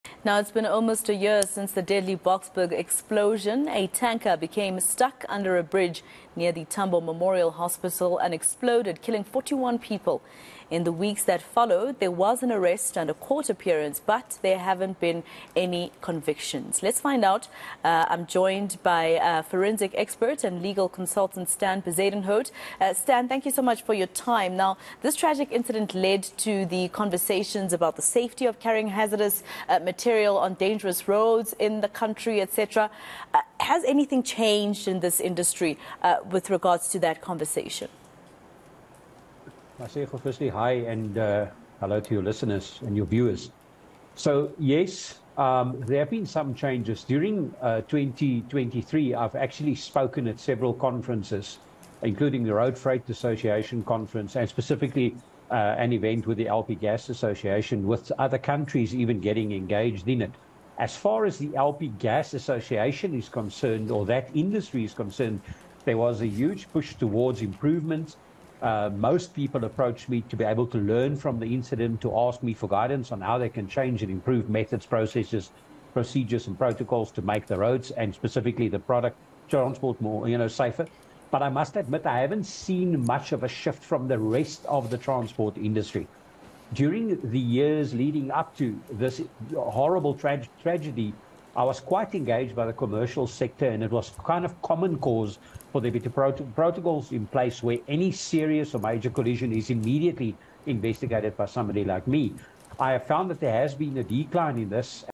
Boksburg Explosion | A year later, horrific accident still haunts the truck industry
It's been almost a year since the deadly Boksburg explosion. A tanker became stuck under a bridge near the Tambo Memorial Hospital, and exploded, killing 41 people. In the weeks that followed there was an arrest and a court appearance, but there have been no convictions.